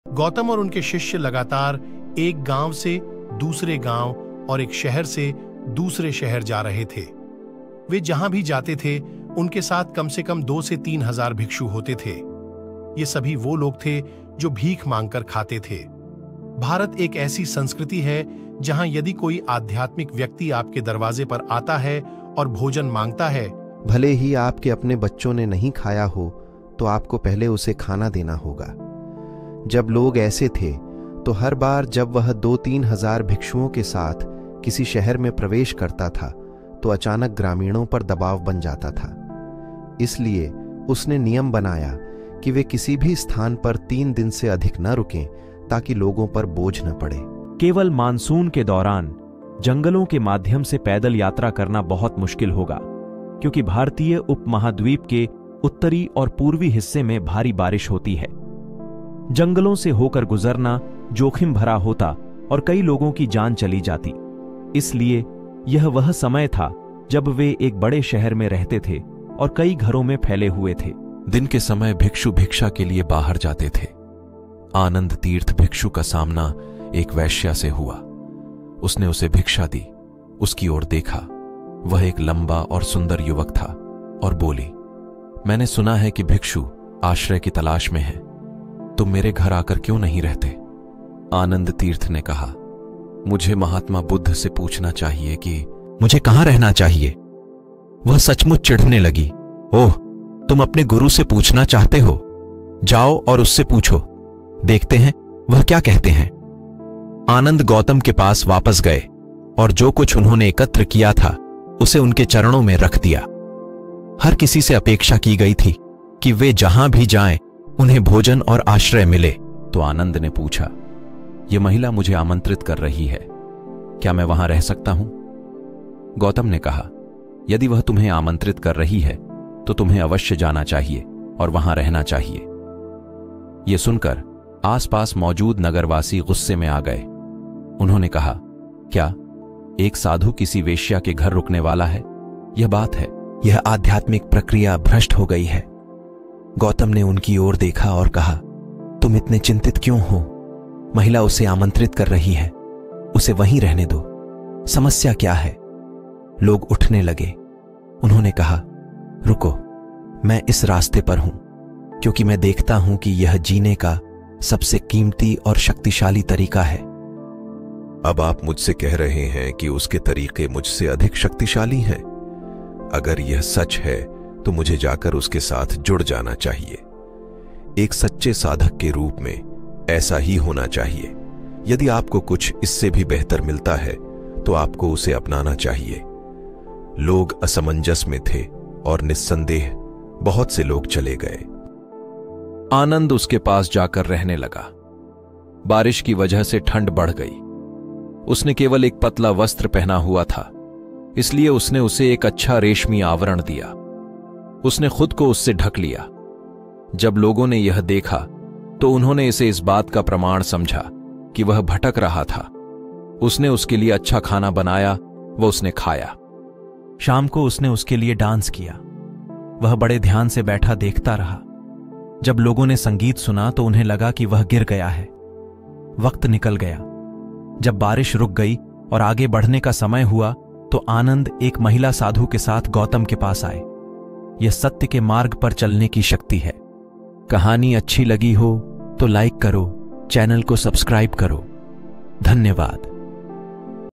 महात्मा बुद्ध ने अपने शिष्य को वैश्या के पास क्यों भेजा। महात्मा बुद्ध की कहानी। Buddha Story
महात्मा बुद्ध ने अपने शिष्य को वैश्या के पास क्यों भेजा। महात्मा बुद्ध की कहानी।  Buddha Motivation Story